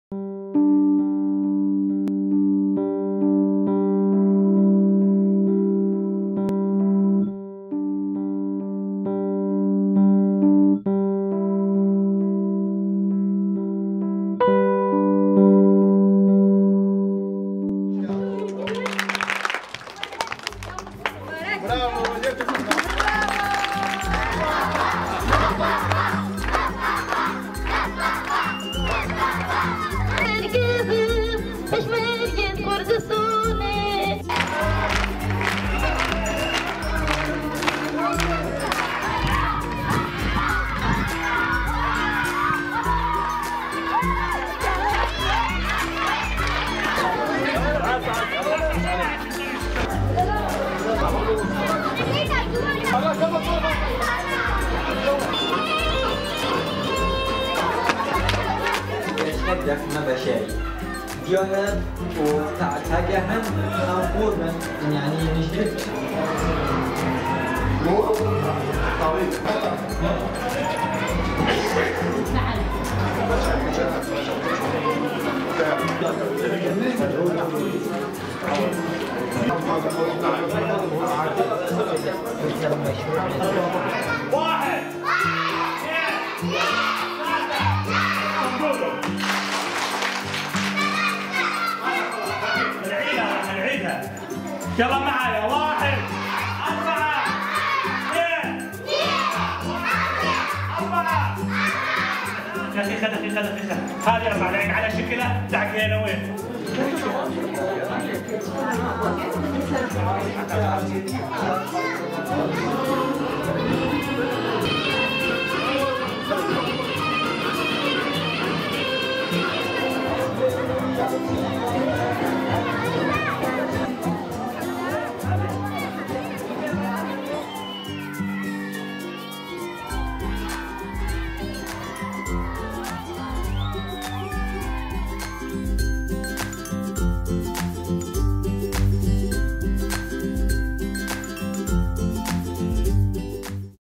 Podpora vzdělávání v Iráku
Část oblasti Ninive v severním Iráku byla nedávno osvobozena od nadvlády tzv. islámského státu. Vysídlení lidé se pomalu začínají vracet do svých vesnic, aby začali nový život. Většina škol je však poničená, a proto se s pomocí UNICEF snažíme opravit budovy, podpořit žáky i učitele a zajistit, aby se děti mohly co nejdříve vrátit do školy, kde se jim bude líbit. Čtěte více na